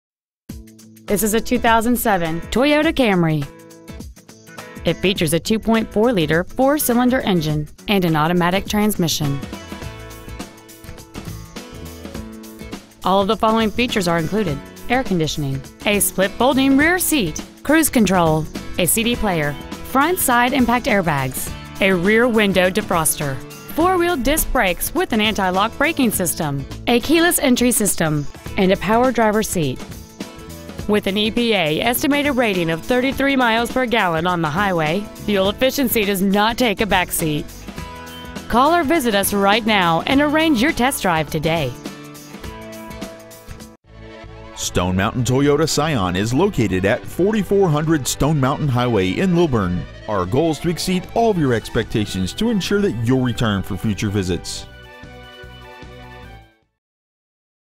Used 2007 Toyota Camry Lilburn GA 30047
We have been honored to serve the Lilburn GA area , we promise that your experience at our dealership will exceed your expectations ! Year : 2007 Make : Toyota Model : Camry Engine : 2.4L I-4 cyl Trans . : Automatic Exterior : Blue Miles : 115,063 Interior : Gray Stock : T7U037857 Stone Mountain Toyota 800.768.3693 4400 Stone Mountain Highway Lilburn , GA 30047